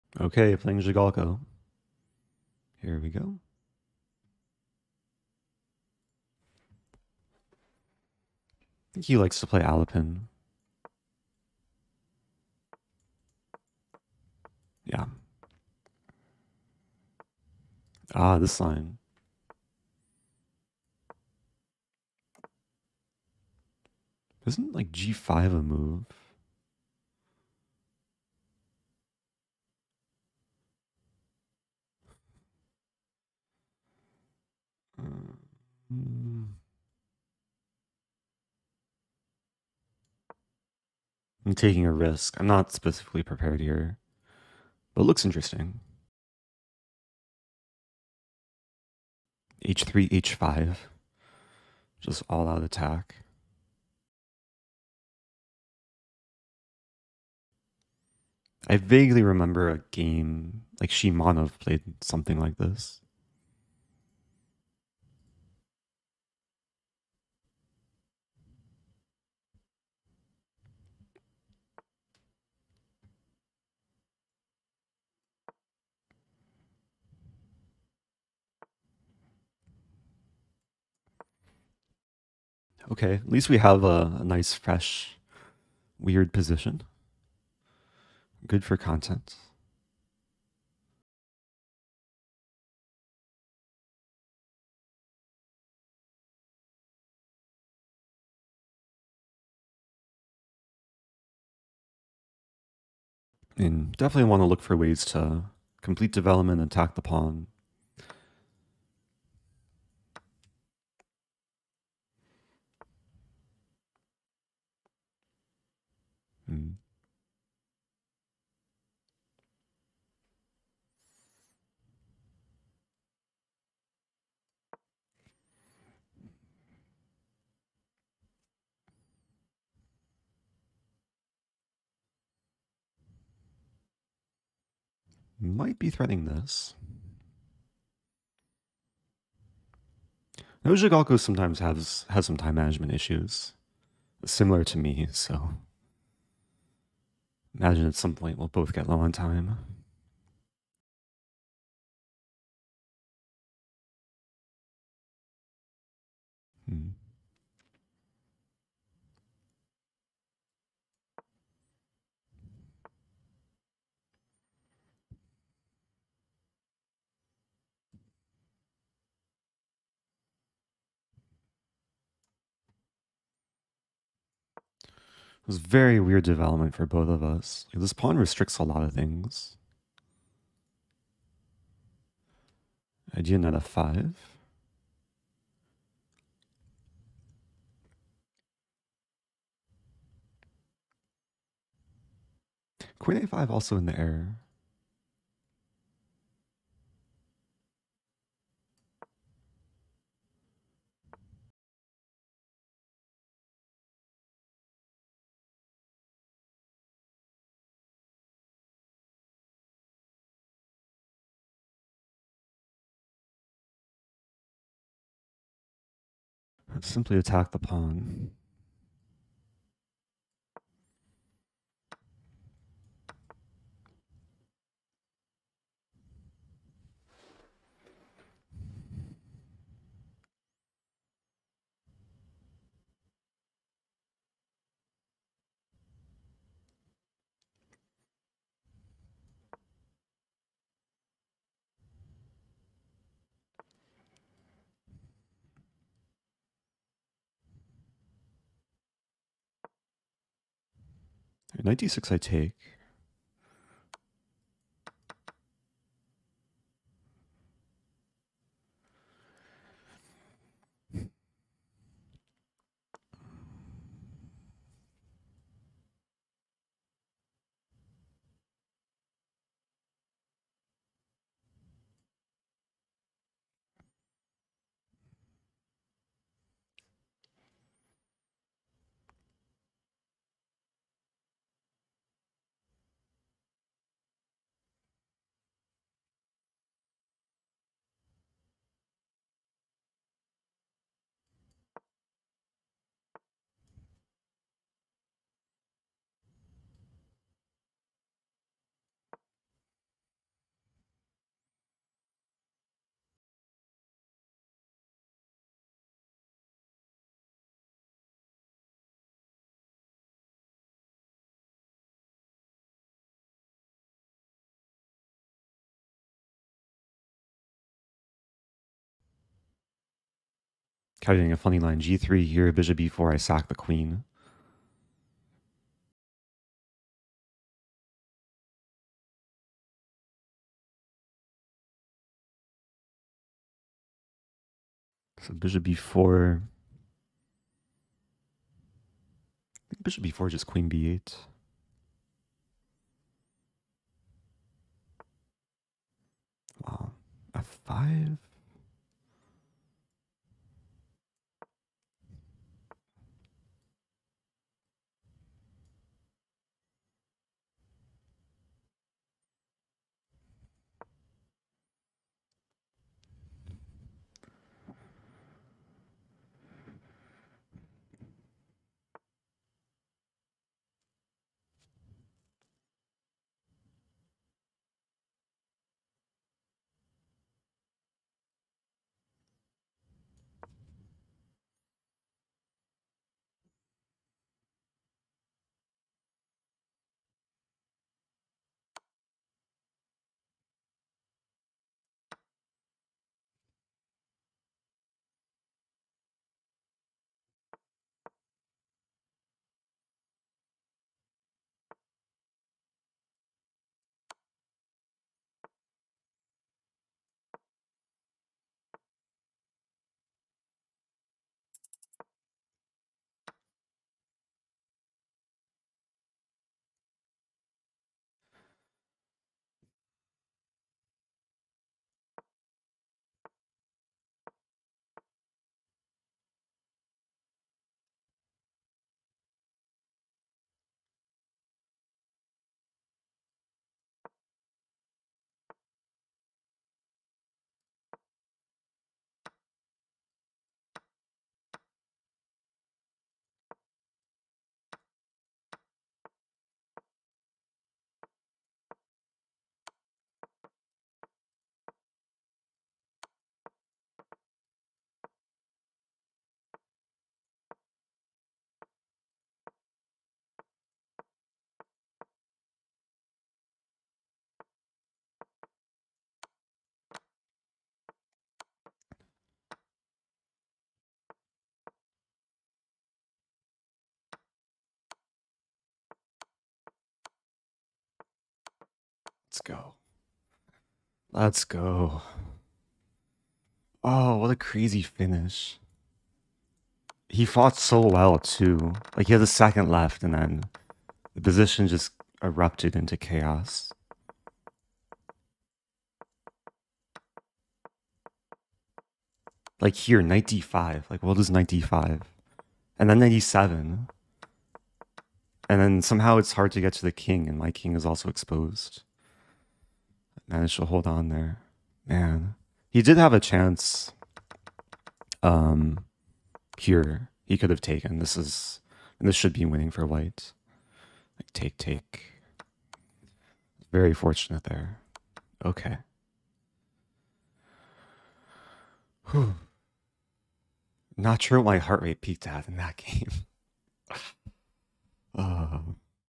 Playing Out of My Mind Against a 2900 Elo Grandmaster
In this video,  I play a fascinating chess game against fellow chess streamer and youtuber,  Grandmaster @SergeiZhigalkoChess. The game featured an exotic idea in the Sicilian Defense followed by an intense middlegame and chaotic ending.

💌SEND FANMAIL ►
Eric Rosen
4579 Laclede Ave. # 205